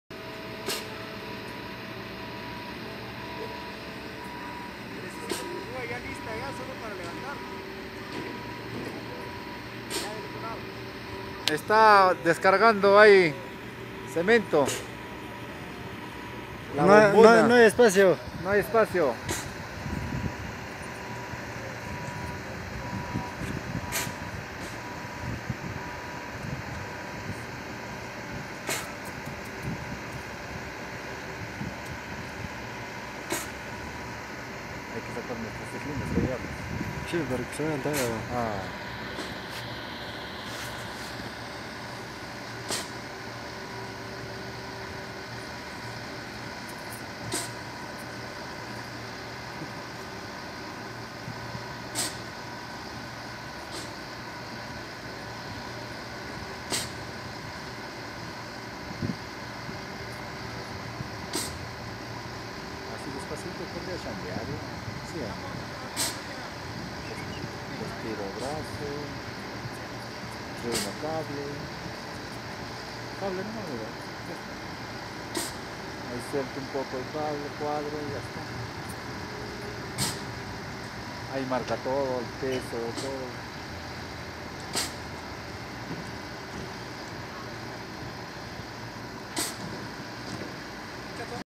Izaje de cilo de cemento
Montaje de cilo de cemento en planta de relleno en paste